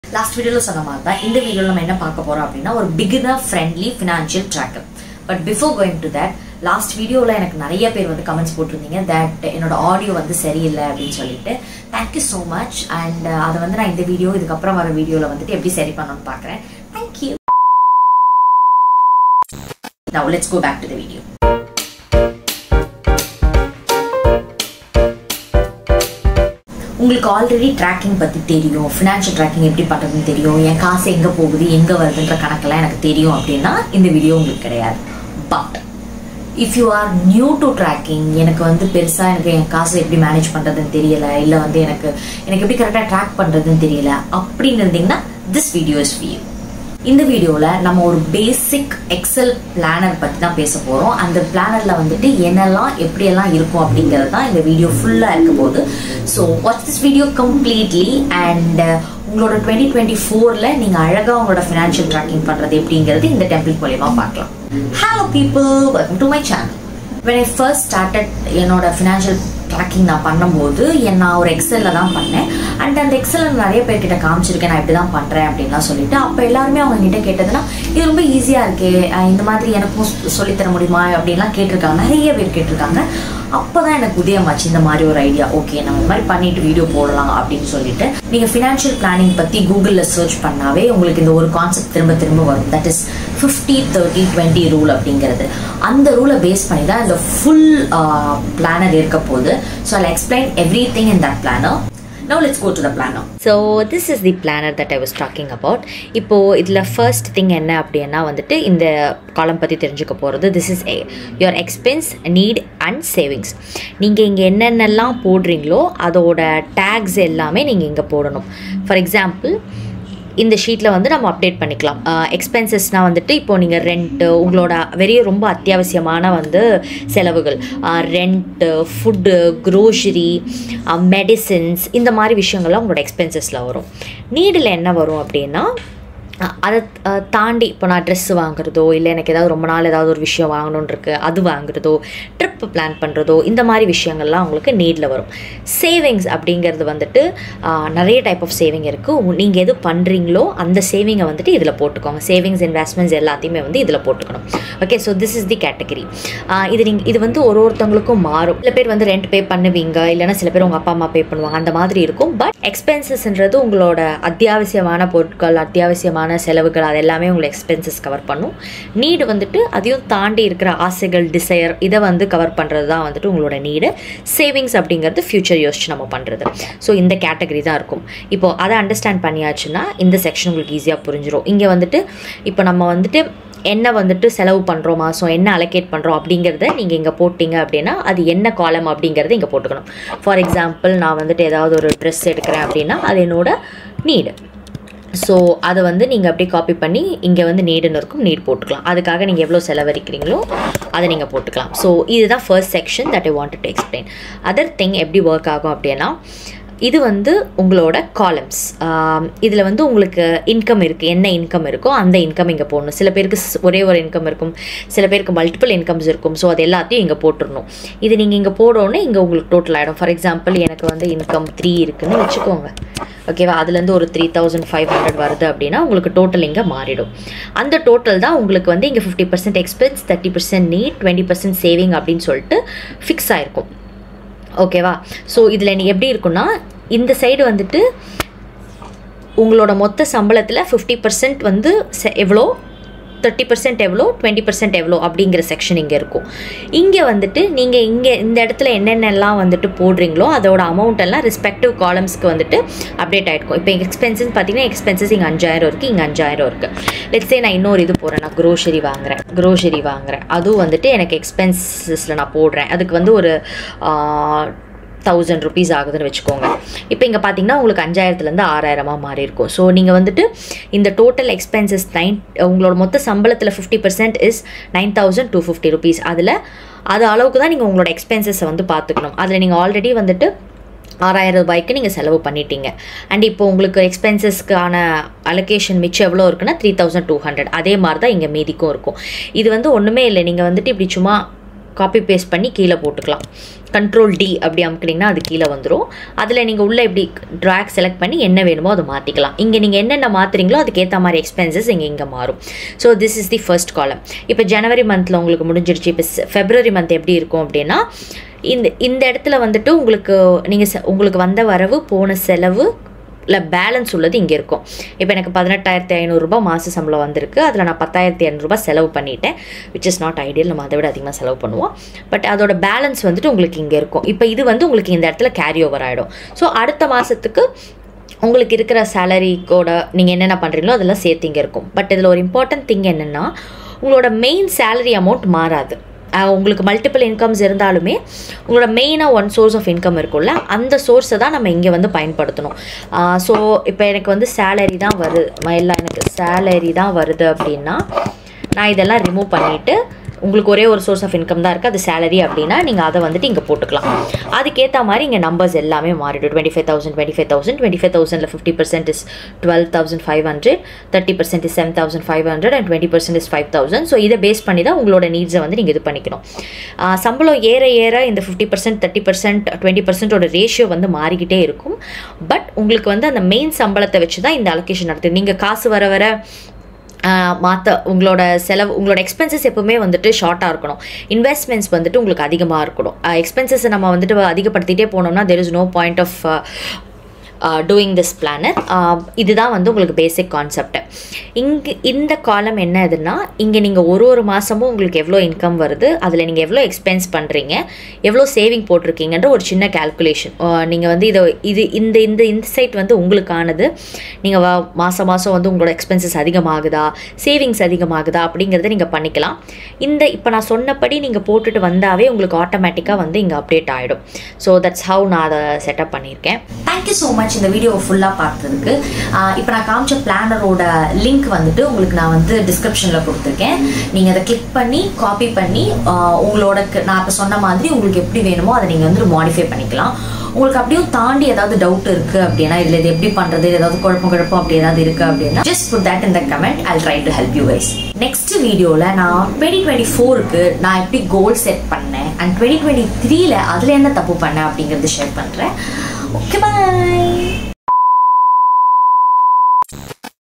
Excel Budget Planner for Beginners | Free Template | Personal Finance | Rya Diaries
Hello people

As I promised, I have shared my beginner-friendly financial tracker.

Free Template link:
Click the link below and copy the file. You will get access to your own file. The file will be in your drive if you wanna access it later.

If you have any doubts, put that in the comment. I will try to help you guys